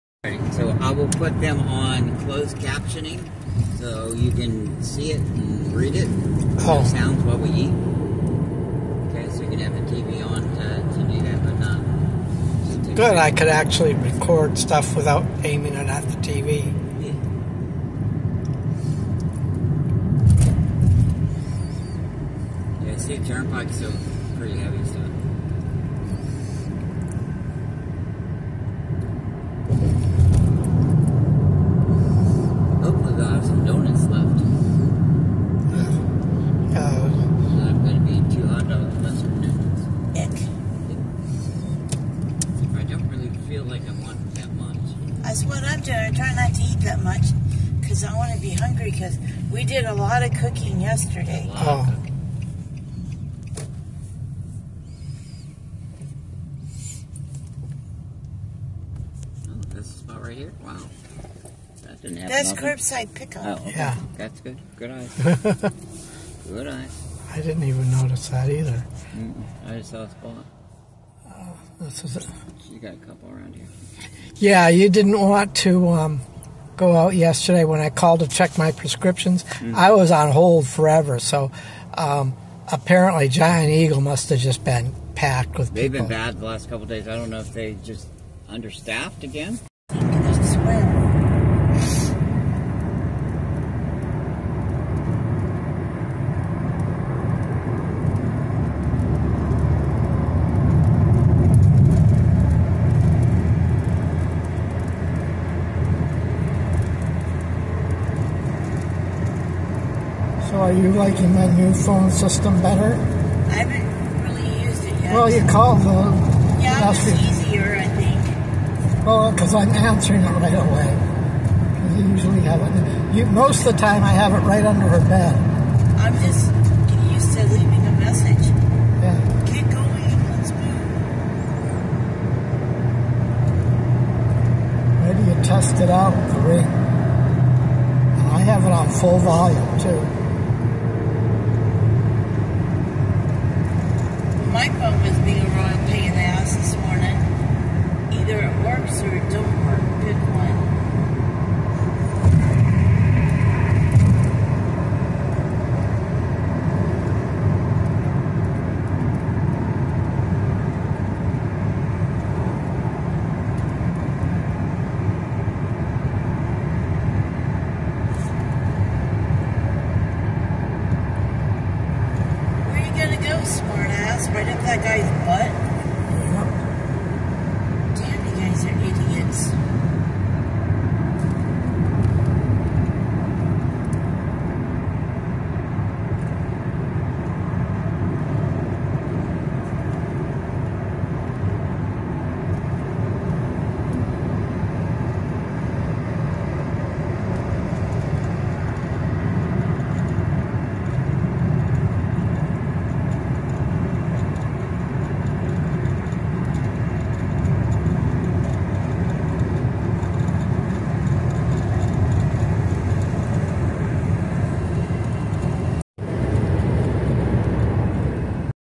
My drive to Thanksgiving dinner before the UGH Lions game
Here is my drive to Thanksgiving dinner with HIGH hopes for Lions. I was so upset after that loss that I actually threw up my TDAY Zfinner so I was NOT filming on my way back home